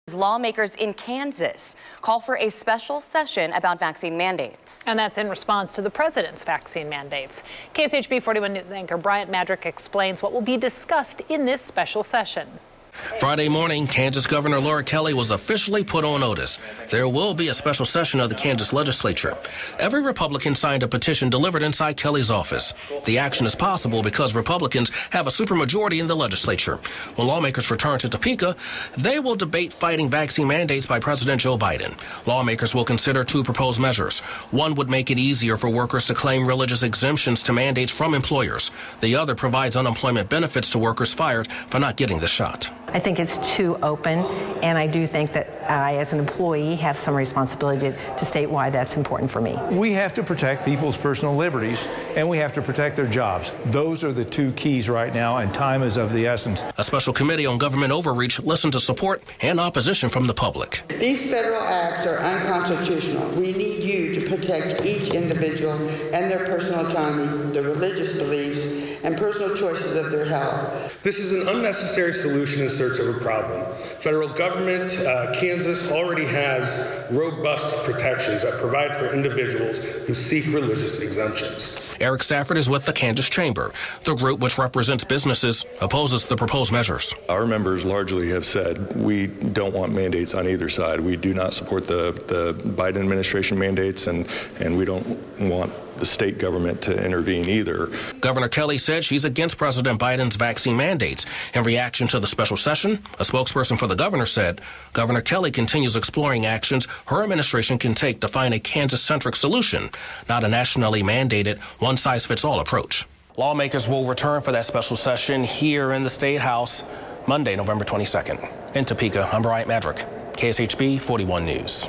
KS legislators call special session on vaccine mandates
Kansas legislators are calling a special legislative session to address President Joe Biden's latest COVID-19 vaccine guidance.